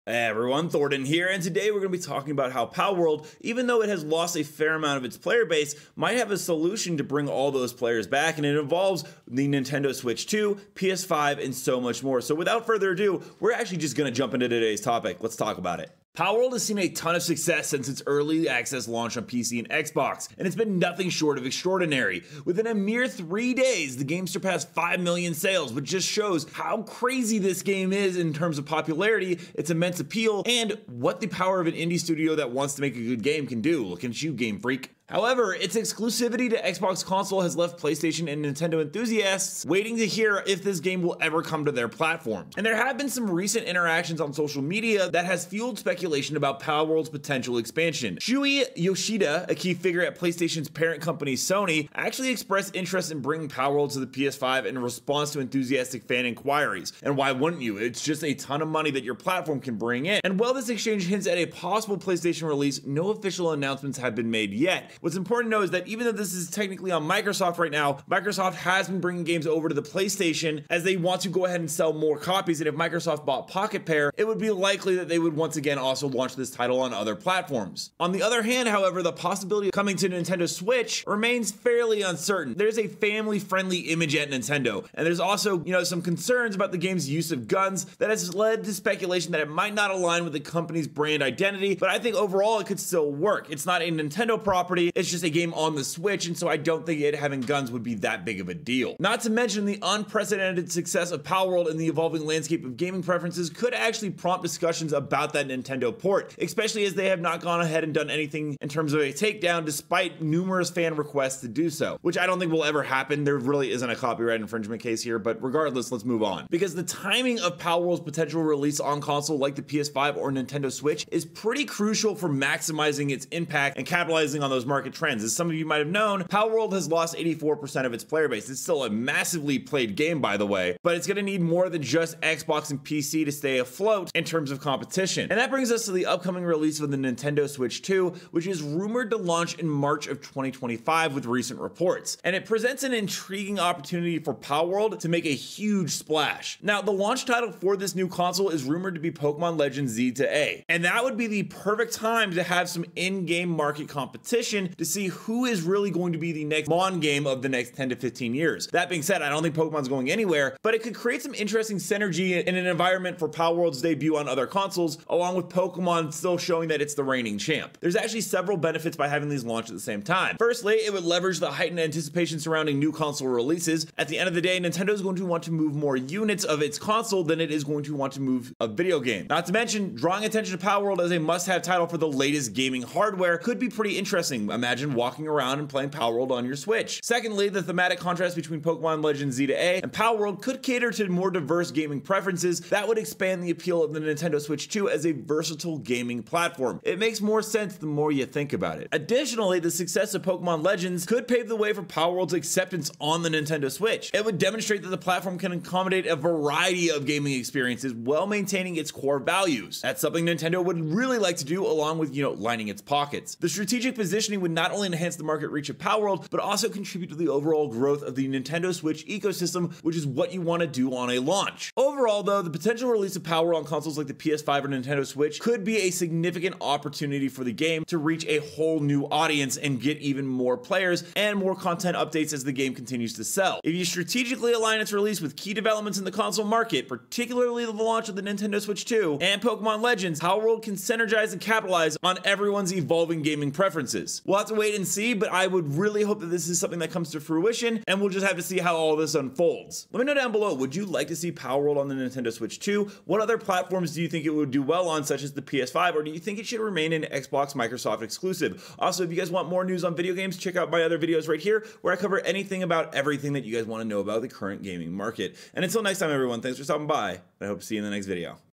Palworlds Nintendo Switch Release Will Be Huge
Palworld Is Coming To Nintendo Switch Sooner Than You Think Palworld has continued to shatter records and expectations, with the hype leaving many wondering if it will come to the PS5, PS4, Switch or Mac to help bump that player count up further. Despite being a few weeks on from launch, Palworld still has the top spot on Steam, according to SteamDB, so it's safe to say the excitement hasn't died down yet, nor does it seem like it will anytime soon.